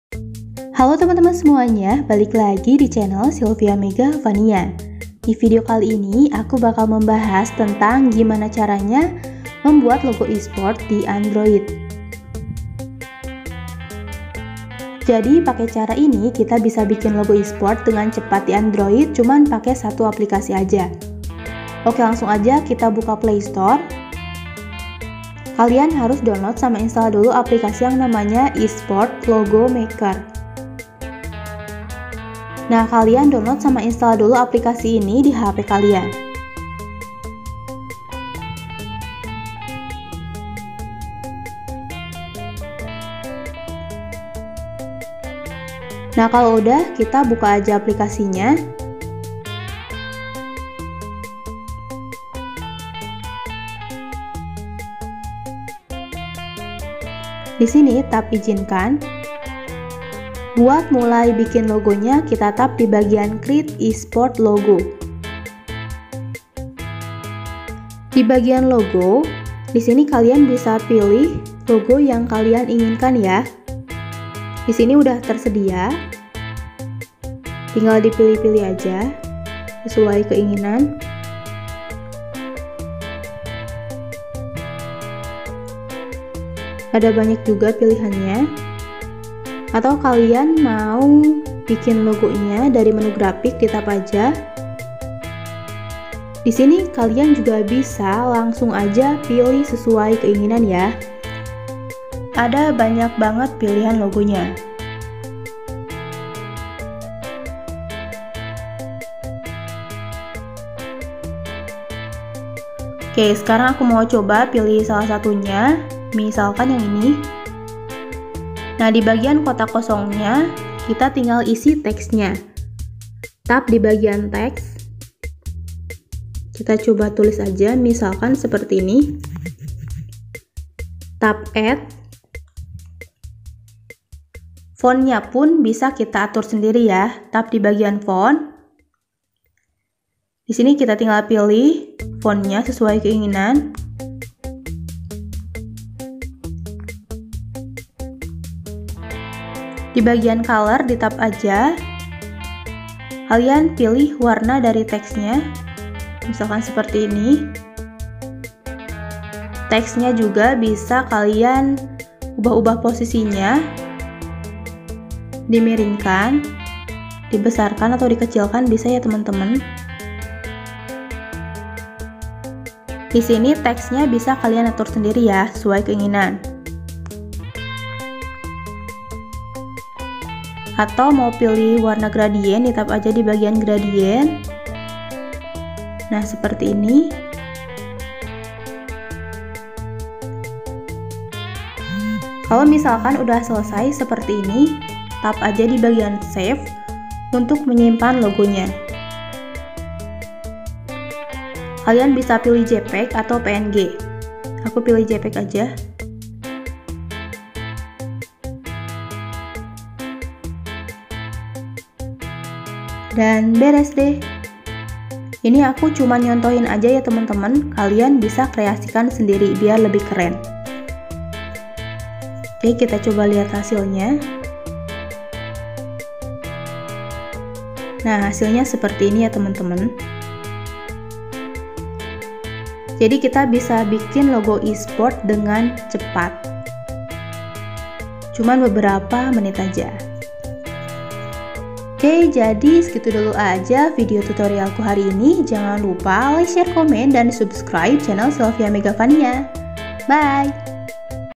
Cara Membuat Logo Gaming Di Android Terbaru
Cara Membuat Logo Gaming Di Android Terbaru - untuk membuat sebuah logo gaming, logo esport, logo squad atau clan kamu bisa membuatnya cukup menggunakan hp android saja tanpa menggunakan komputer. Di video kali ini saya akan share cara simpel untuk bisa membuat logo gaming dengan cepat dan mudah hanya menggunakan 1 aplikasi saja. Selengkapnya lihat di artikel berikut ini